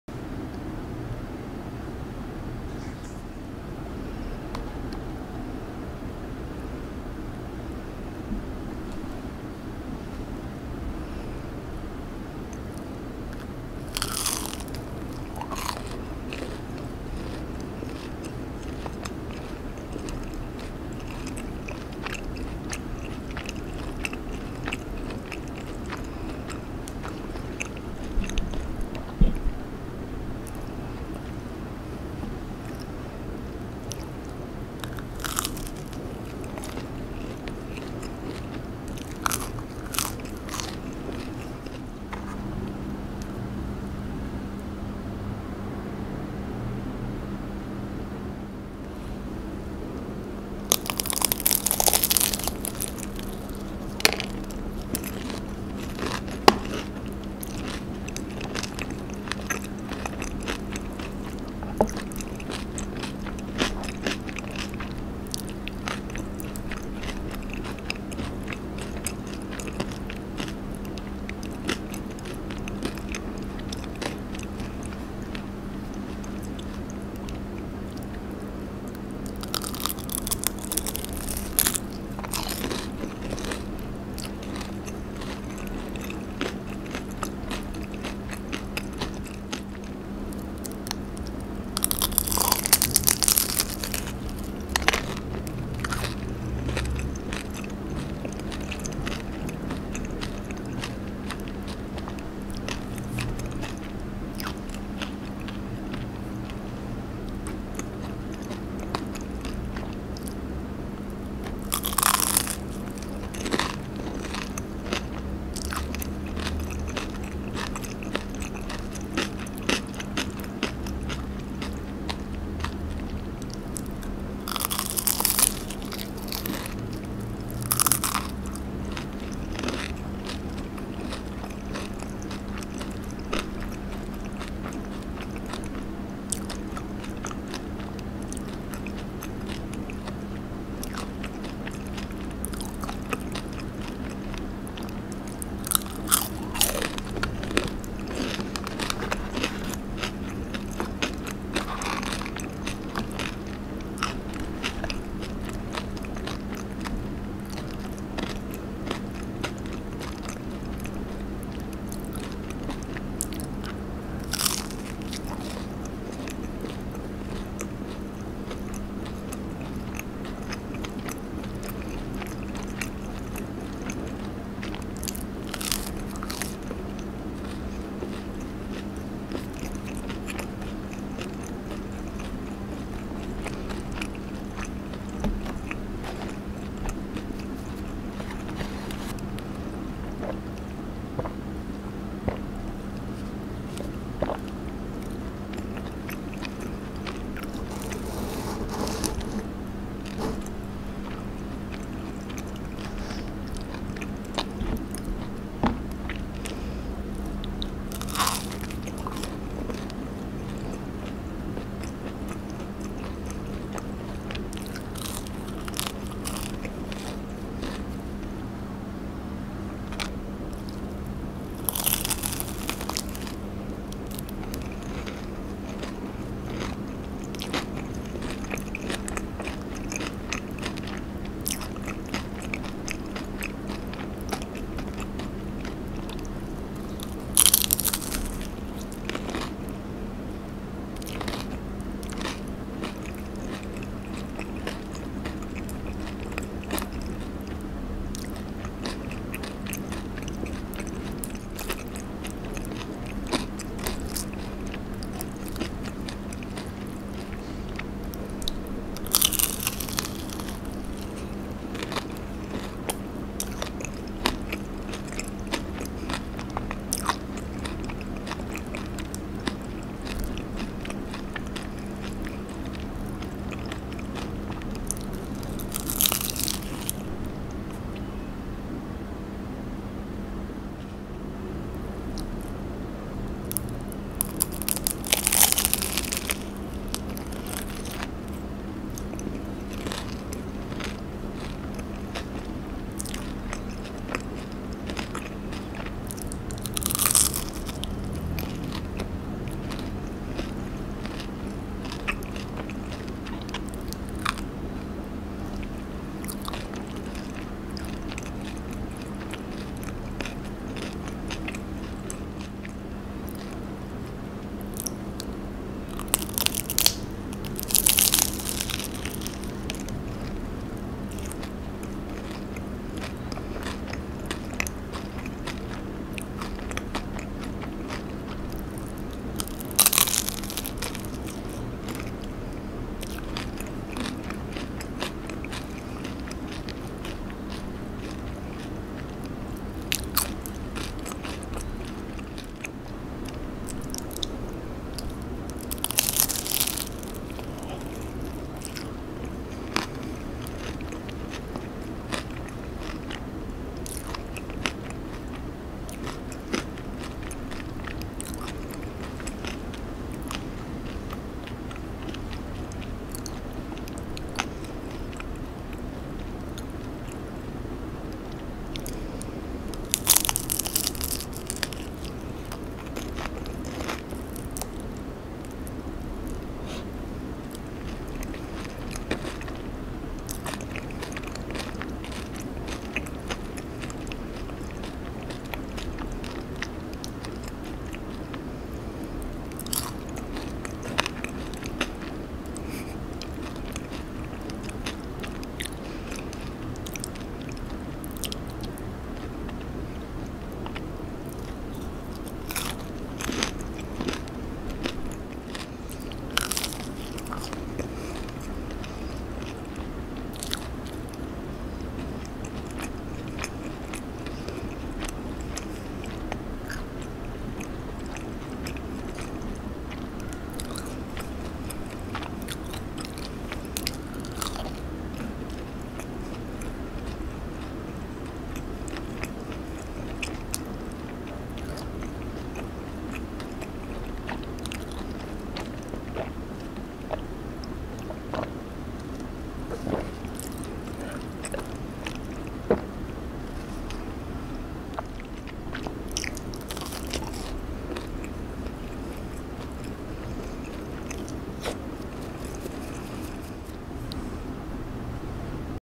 ASMR EXTREME CRUNCH McDonald's New Pies | Eating Sound | *No Talking*
NEW McDonald's Salted Caramel & Chocolate ! I also have Mini Corn Pie, Mini Apple Pie and McNuggets. It is so satisfying eating crunch and crispy food.

Let’s Connect

XOXO